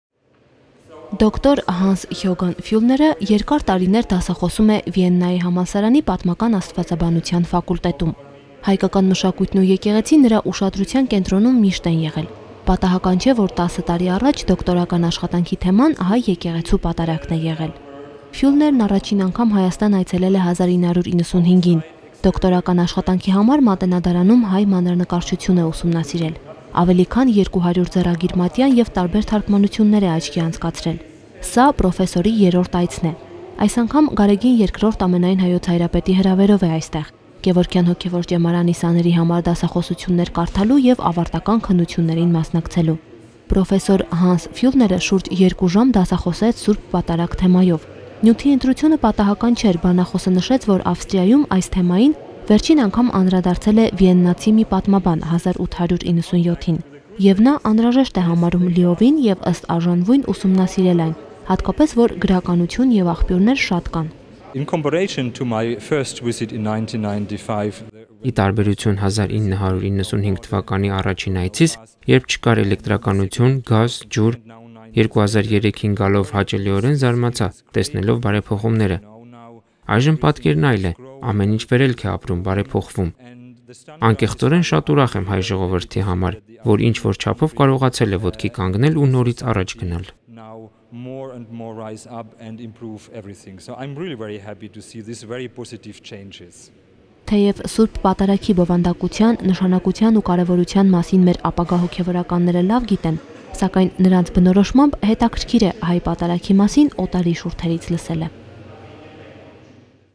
Dr. Hans-Jurgen Fyulner's lecture in the Gevorkian Theological Seminary
Dr. Hans-Jurgen Feulner, Executive Director and Professor of Liturgical Studies and Sacramental Theology at the Institute of Historical Theology at the University of Vienna, visited the Mother See of Holy Etchmiadzin and served as a guest lecturer at the Gevorkian Seminary.

Դոկտոր Հանս-Յուրգեն Ֆյուլների դասախոսությունը Գևորգյան հոգևոր ճեմարանում.

Վիեննայի համալսարանի Պատմական աստվածաբանության ինստիտուտի պատարագագիտության և հաղորդության աստվածաբանության գործադիր տնօրեն և պրոֆեսոր դոկտոր Հանս-Յուրգեն Ֆոլներն այցելեց Մայր Աթոռ Սուրբ Էջմիածին և որպես հրավիրյալ դասախոս Գևորգյան ճեմարանում: